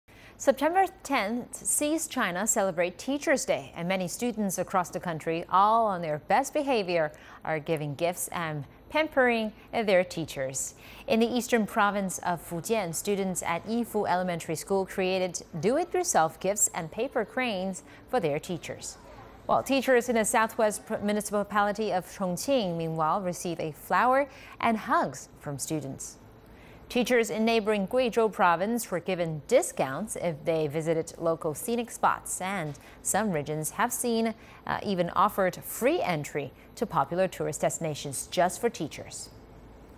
Special treatment for teachers in China
China marks Teachers’ Day on September 10 of every year, and this year students across the country presented gifts to their teachers and pampered them as a sign of recognition to all the efforts they put into their work. In the eastern province of Fujian, students at Yifu Elementary School created do-it-yourself gifts and paper cranes for their teachers. Meanwhile, teachers in the southwest municipality of Chongqing, received a flower and hugs from students.